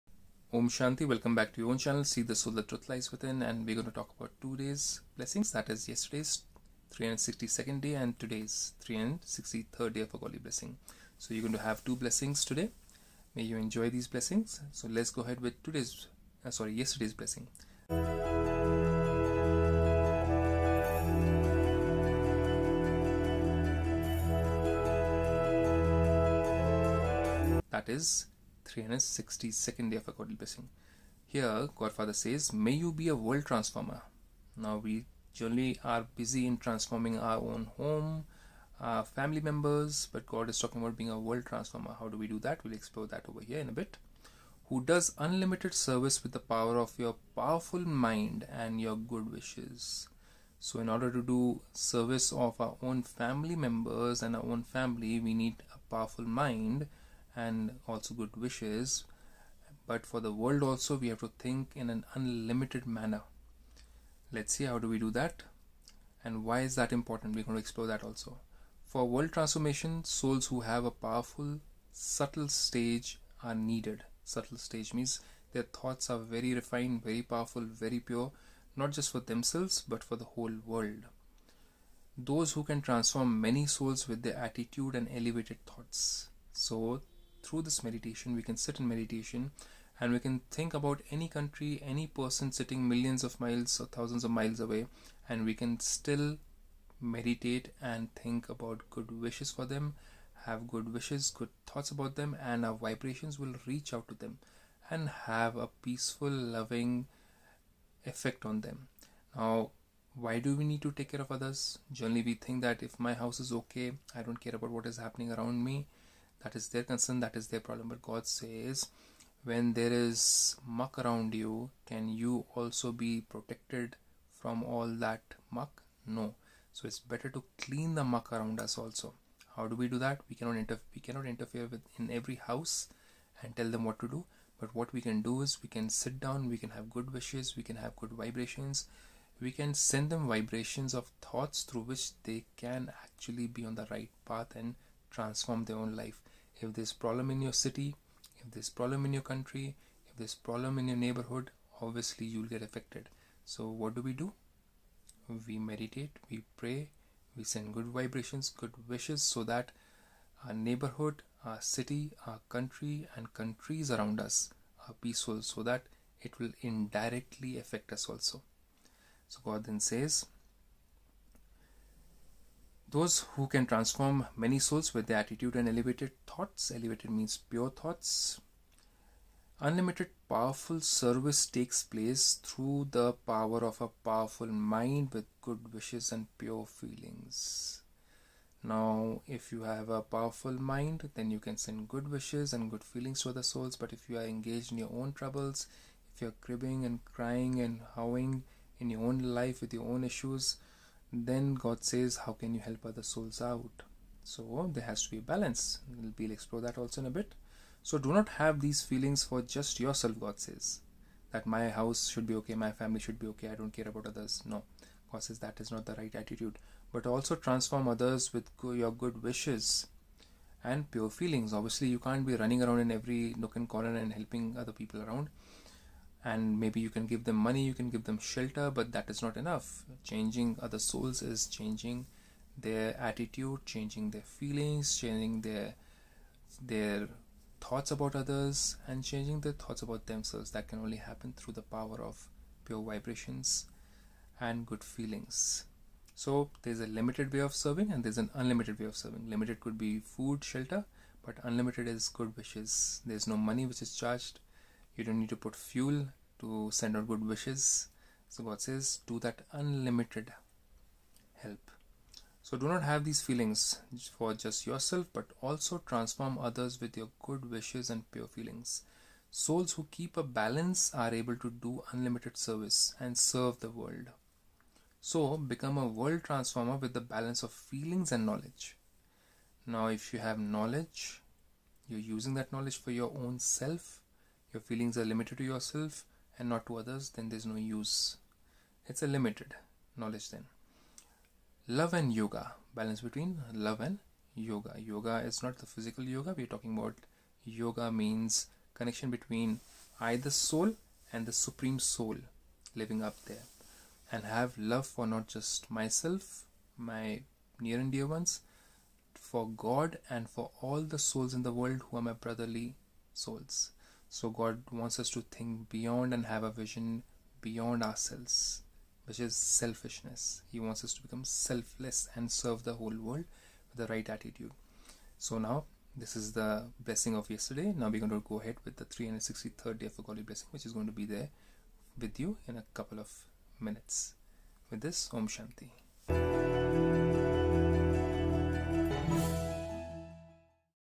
Serving the world is possible while sitting at home ! How ?
Thinking only about our own family and ignoring what happens around us , in our neighbourhood , in our city or our neighbouring countries doesn't make us safer or comfortable . Just like one can't just feel clean by ignoring the muck around one's home , even though the home is clean . It does impact the home . Thus it is time to become a world transformer spiritually and do unlimited form of service through meditation with good wishes & a powerful mind . Even our thoughts can impact world peace from many miles away . Let us find out how ?

Om Shanti .